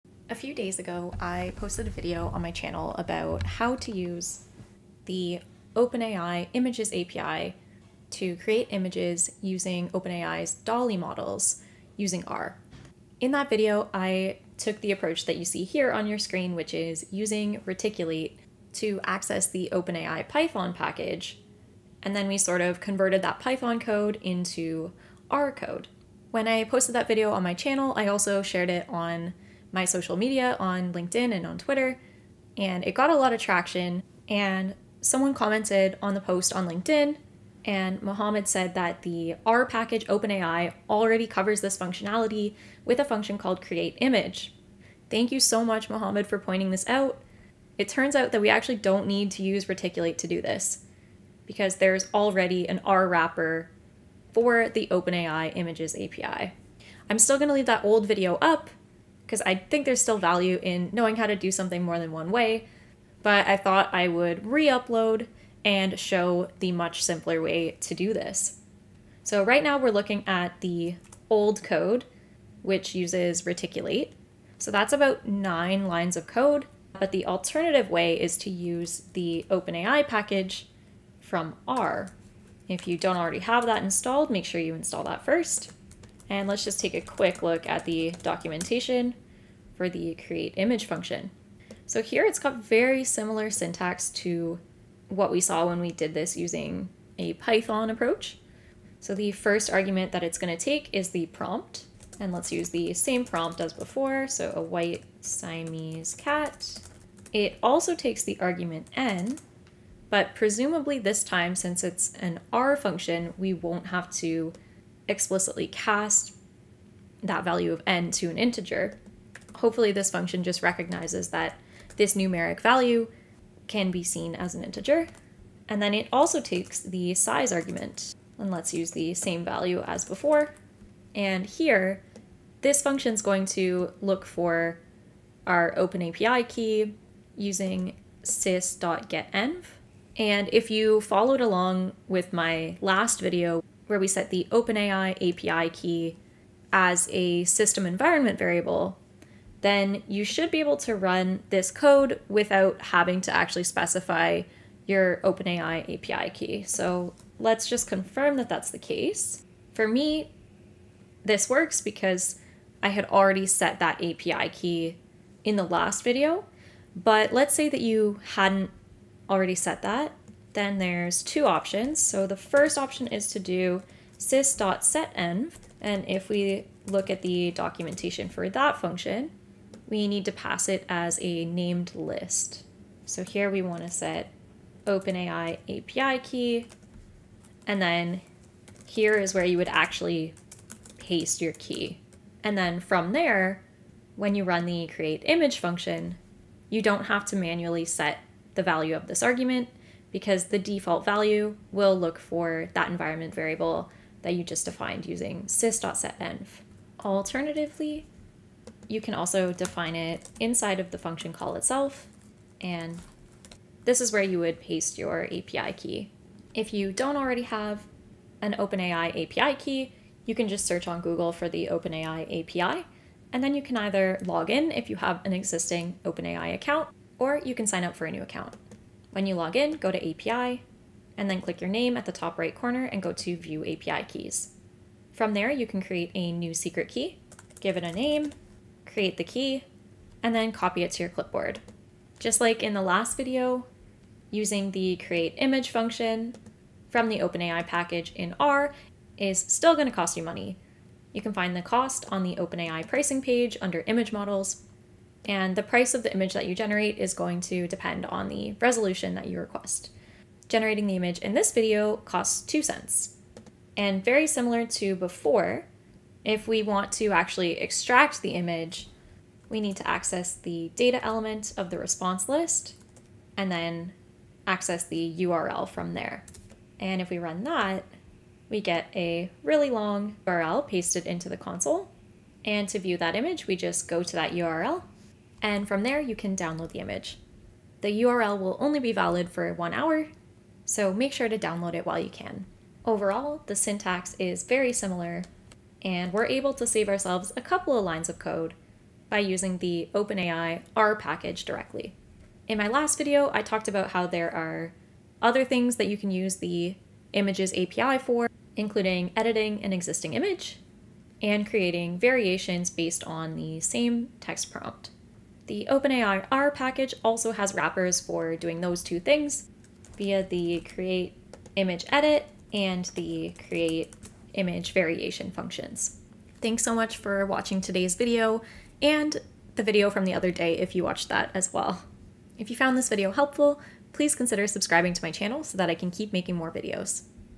How to generate images with OpenAI's DALL-E models using R - openai R package approach (new)
In this video, we learn how to use the OpenAI Images API to generate images based on text prompts using OpenAI's DALL-E models. We do all of this using the openai R package, rather than using a reticulate approach.

This video is a simplified version of a video I posted the other day (which uses the openai Python package via reticulate)

The code from this tutorial is available on my GitHub

*Please note: it’s helpful for me if you send me a message on LinkedIn when you add me letting me know you’re adding me because of YouTube, so that I can keep track of how I know each person in my network😊